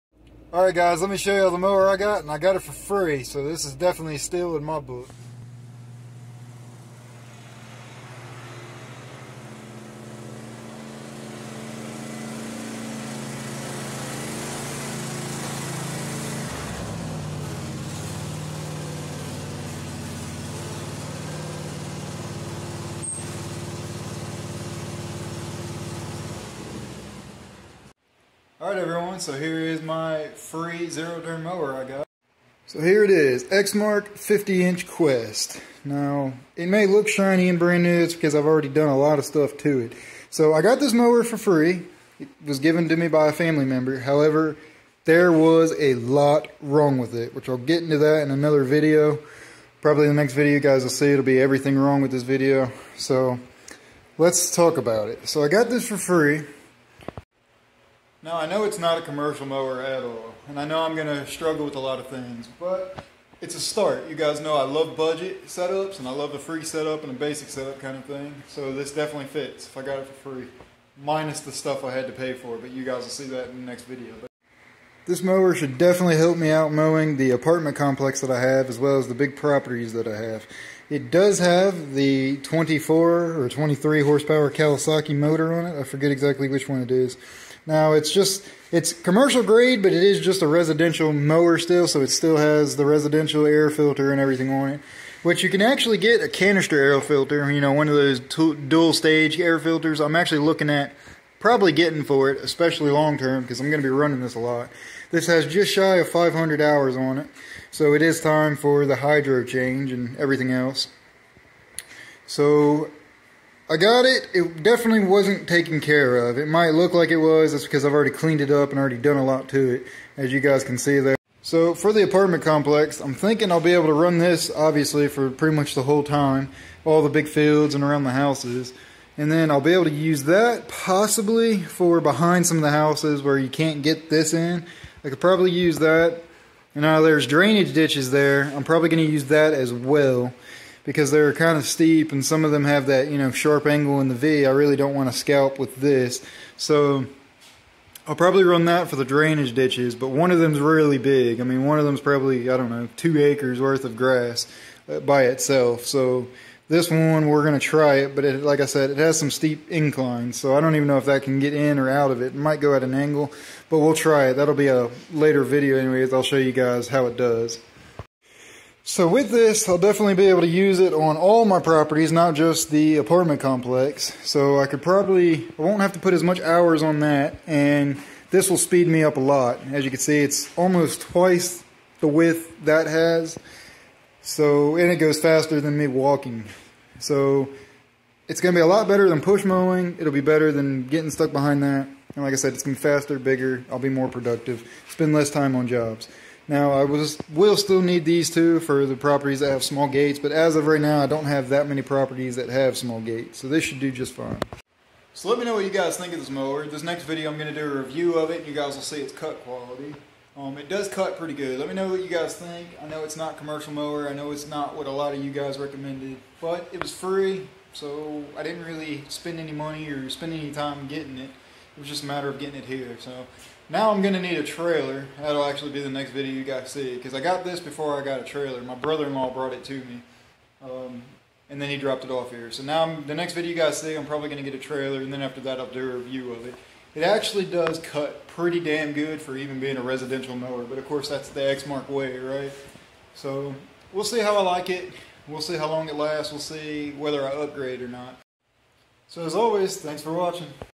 I Got A Free Zero Turn - Lawn Care Vlog
Welcome to Jon's Daily Hustle. I got a Free Zero turn lawn mower for my lawn care business.